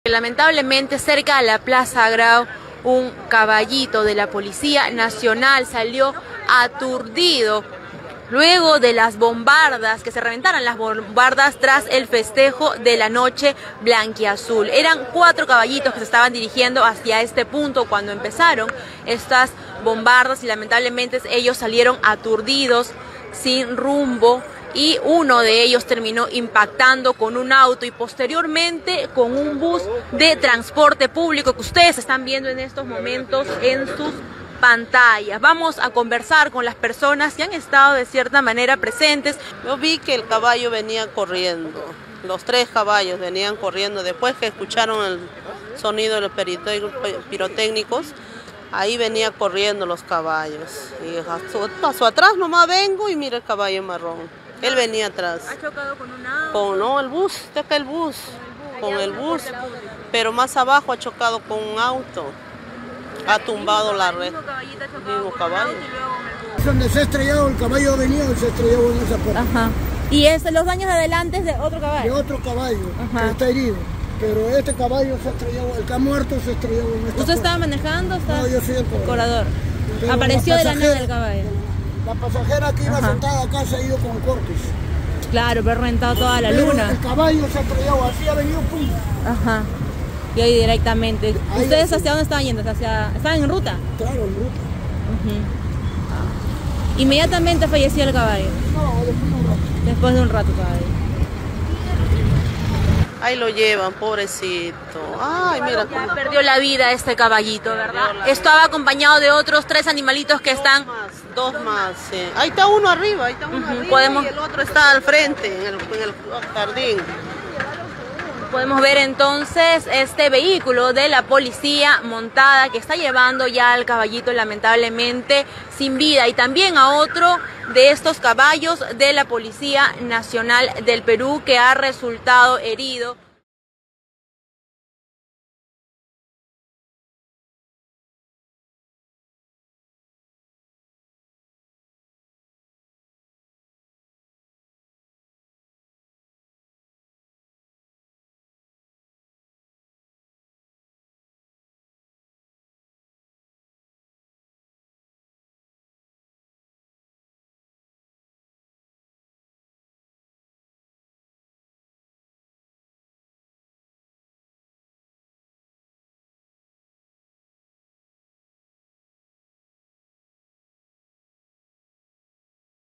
Noche Blanquiazul: CABALLO de la PNP muere tras CHOCAR contra VEHÍCULO
Un caballo de la Policía montada del Perú falleció luego de que chocara con un bus de transporte público después de huir por el ruido de las bombardas y fuegos artificiales durante la Noche Blanquiazul en el Estadio Nacional.

▶️ Alcalde de ANCÓN anuncia CIERRE DE PLAYAS no saludables tras 2 años de DERRAME DE PETRÓLEO